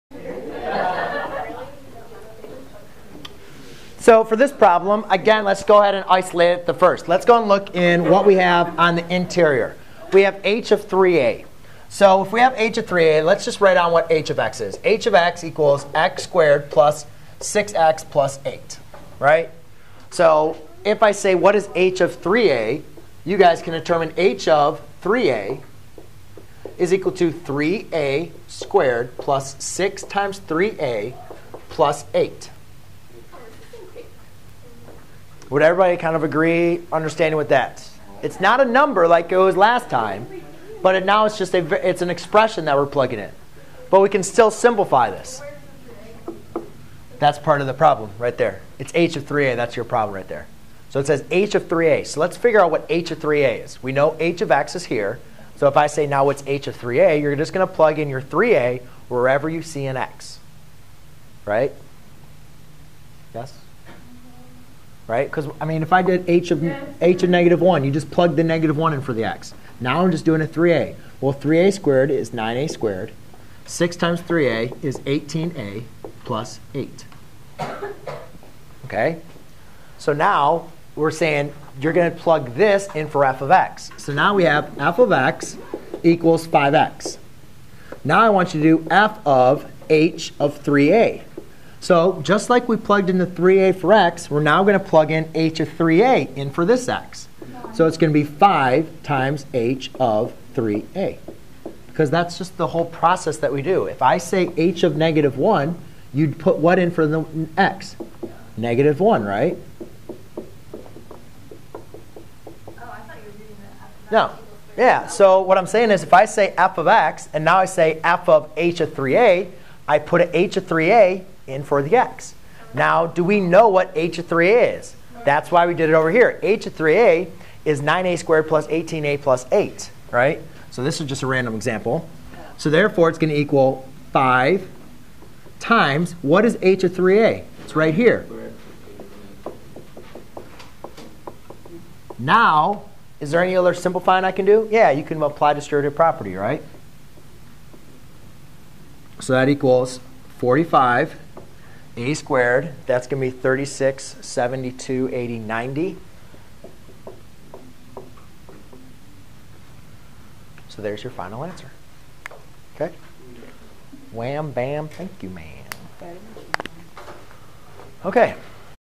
Evaluate with a Variable the Composition of Two Functions
Learn how to compose two functions where one or both of those functions is/are quadratic. To compose two functions means to express one of the functions as a function of the other function. This is done by replacing the input variable of one of the functions with the value of the second function. We will then simplify the composition and determine the domain.